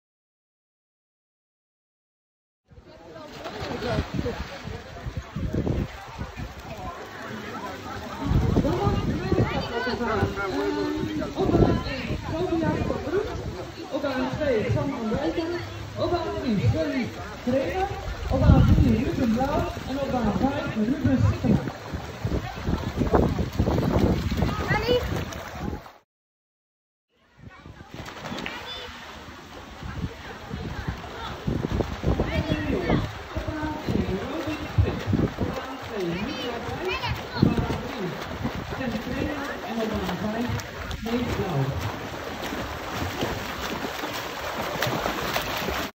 Mandy’s swimming log
Swimming journey. Way to the national swimming team. Dream big.⭐️
Young Dutch swimmer clocked two new personal records despite less training during the pandemic.